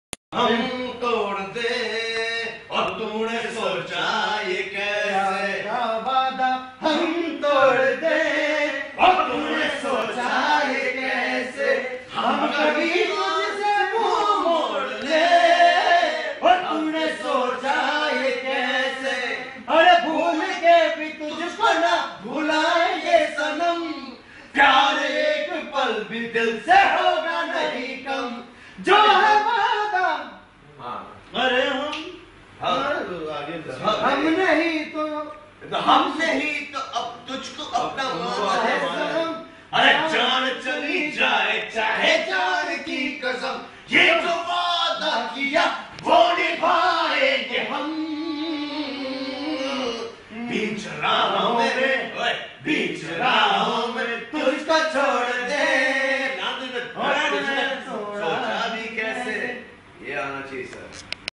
wajid khan with salman khan singing | During veer movie shooting
Salman khan drunk and singing loudly